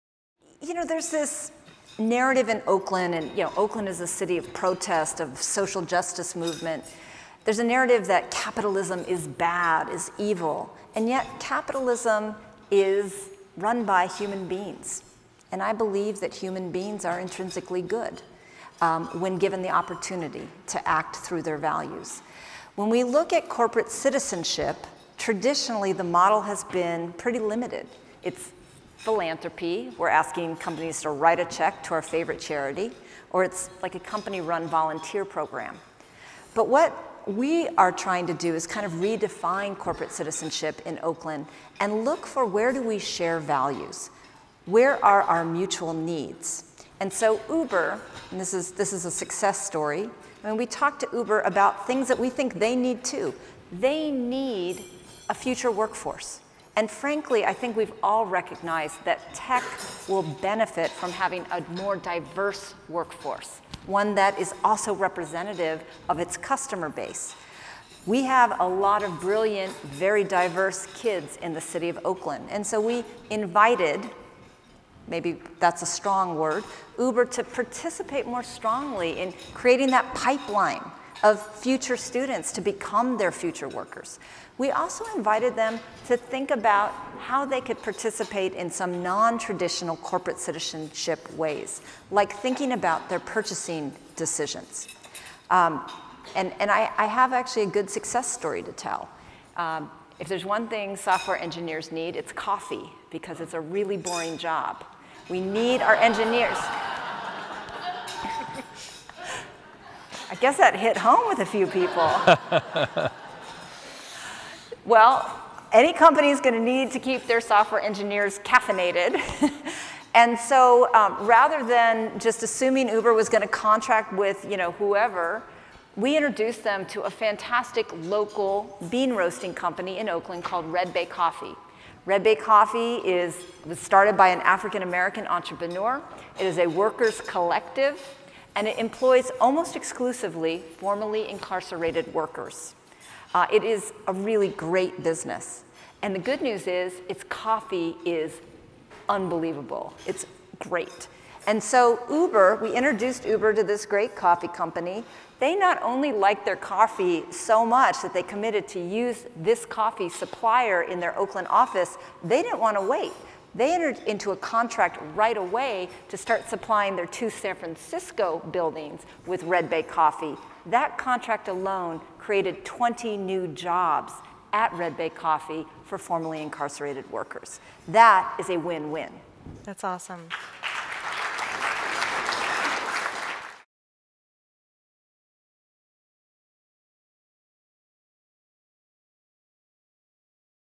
What Makes a Great City? - Libby Schaaf (City of Oakland)
Our special dinner guest will be Libby Schaaf, mayor of Oakland. As mayor, Libby has focused on forward-looking policies that have made Oakland one of the best places to live in the country. It’s a vibrant, multicultural city that is navigating the transition to becoming a tech hub. Uber is moving its headquarters to Oakland, as are other technology companies, and its easy access to San Francisco has made it a home of choice for many who work in San Francisco and points south. We’ll talk with Libby about Uber’s move. How is Oakland planning to navigate the forces of gentrification? How can the city welcome economic growth without pricing out long-time residents and the intense local flavor that makes Oakland so special? We’ll also talk with Libby about some of her key initiatives: Oakland Promise, an effort to triple the number of low-income Oakland residents who receive a college degree, her forward-looking Department of Transportation, which aims to develop policies to reshape transportation in the city in line with the possibilities of the 21st century, her affordable housing plan, and efforts to improve police-community relations.